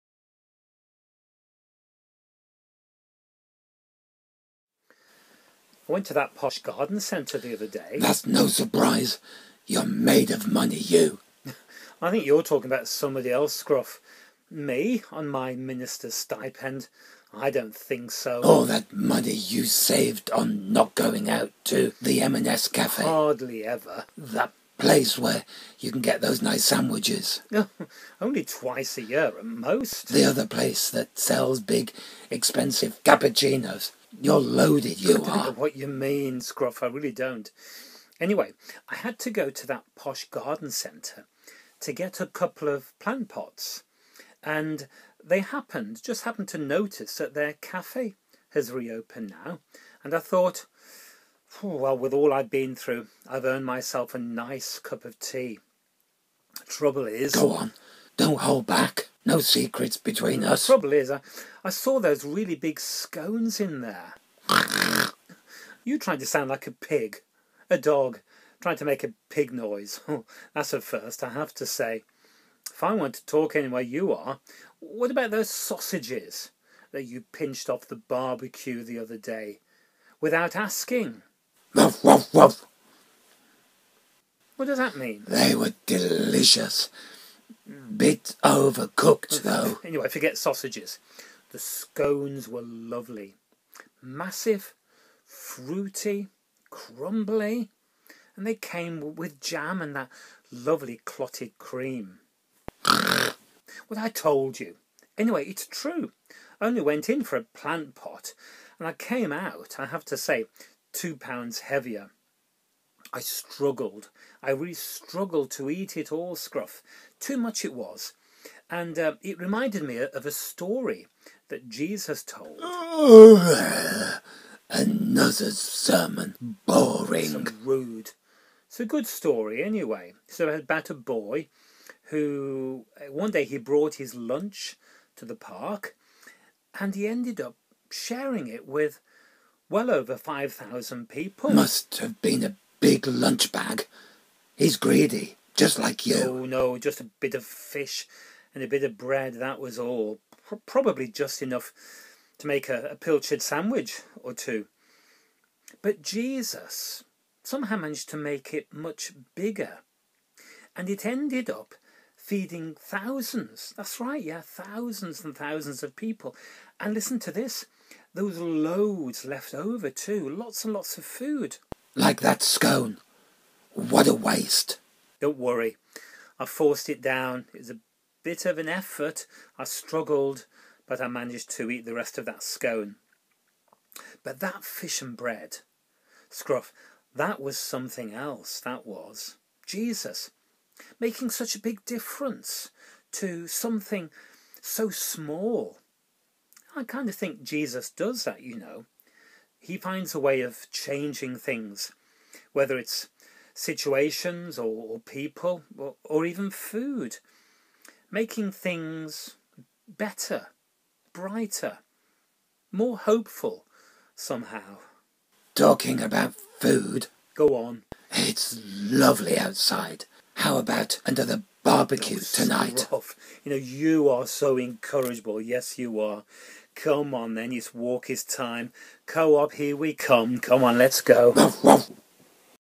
Scruff and Darren talk about...food
Scruff and Darren have a little chat about posh cafes, sausages and a little boy's lunch box of bread and fish.